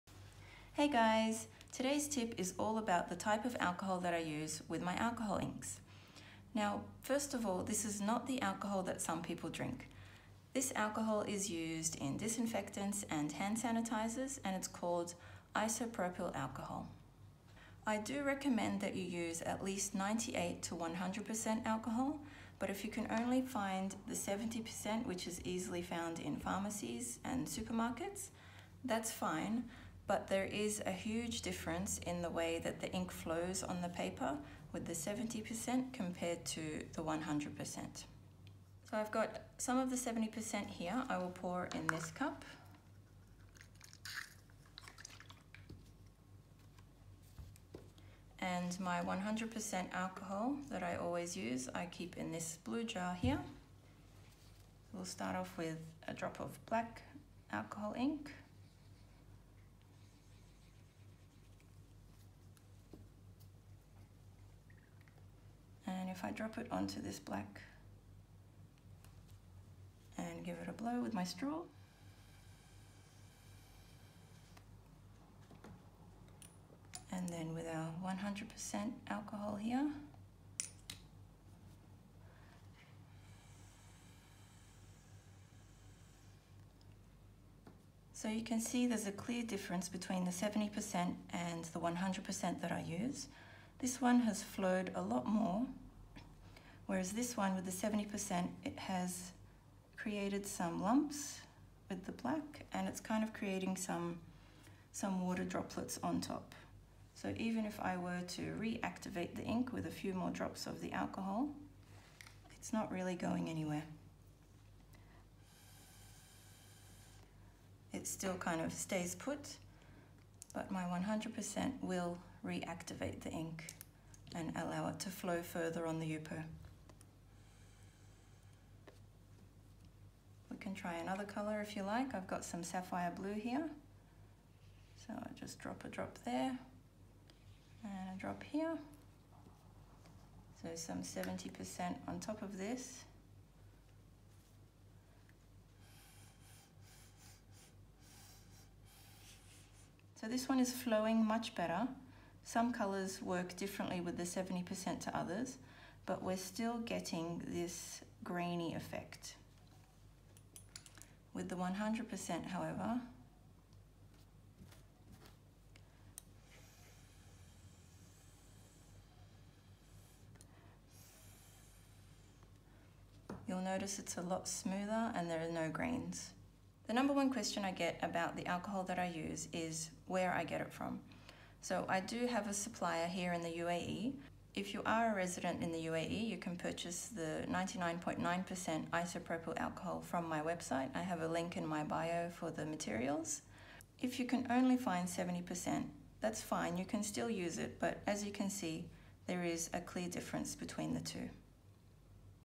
Which Isopropyl Alcohol to use with Alcohol Inks
The difference between 70% & 100% Isopropyl Alcohol.